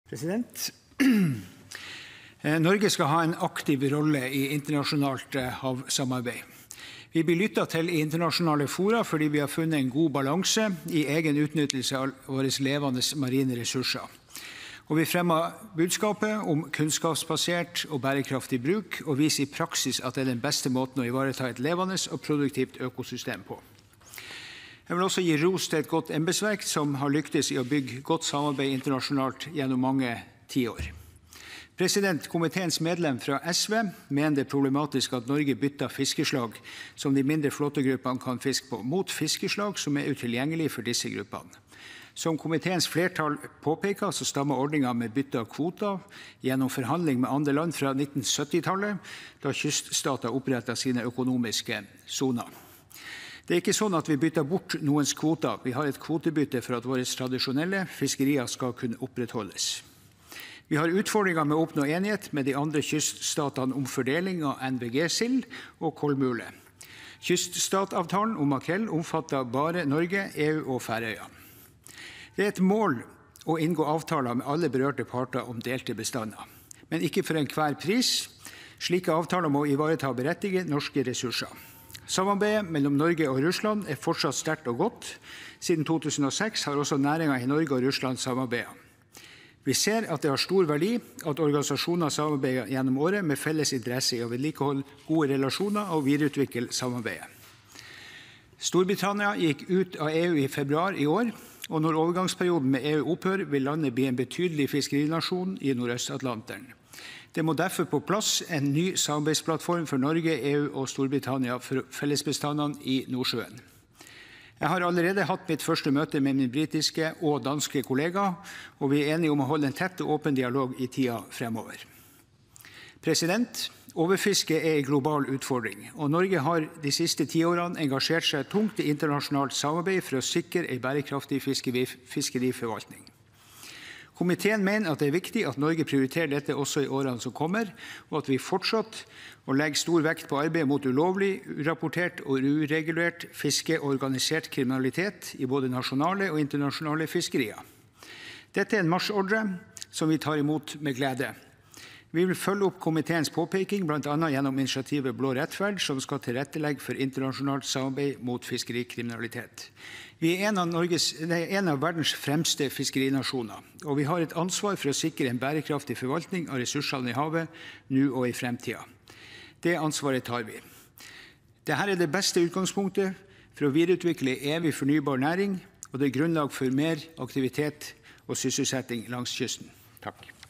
26.05.2020 Sak 22 1 Odd Emil Ingebrigtsen
Om Noregs fiskeriavtalar for 2020 og fisket etter avtalane i 2018 og 2019 (Innst. 299 S (2019–2020), jf. Meld. St. 13 (2019–2020))